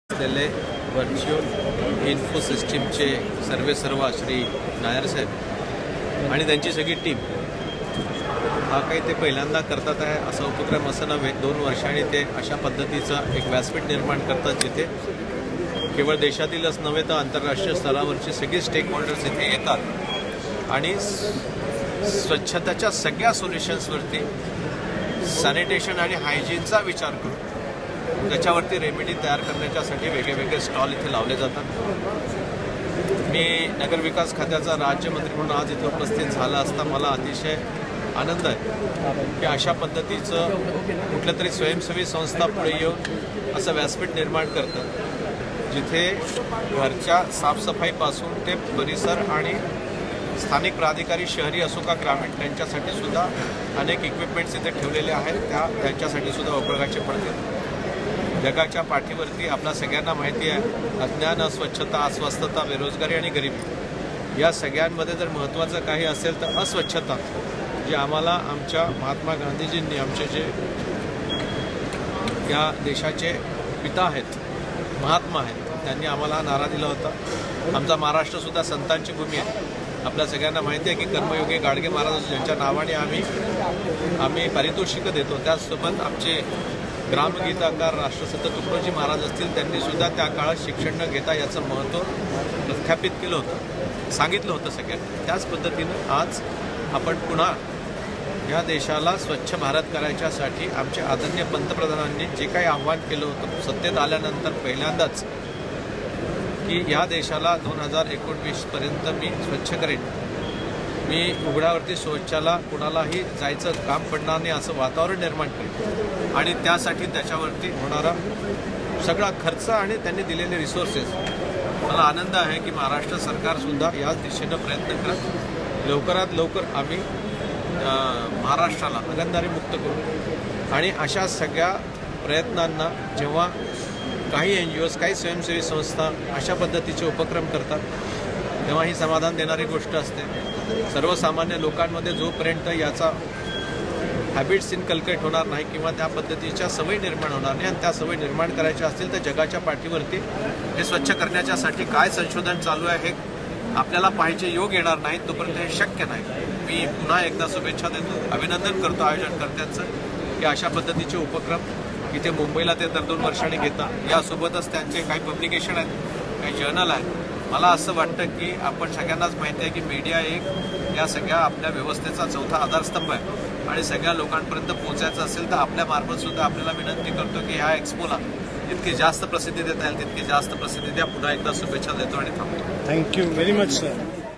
Interview with Dr Ranjit Patil, Minister of State, Urban Development,Govt of M
Dr Ranjit Patil, Minister of State, Home (Urban), Urban Development, Government of Maharashtra  at Clean India Technology Week 2018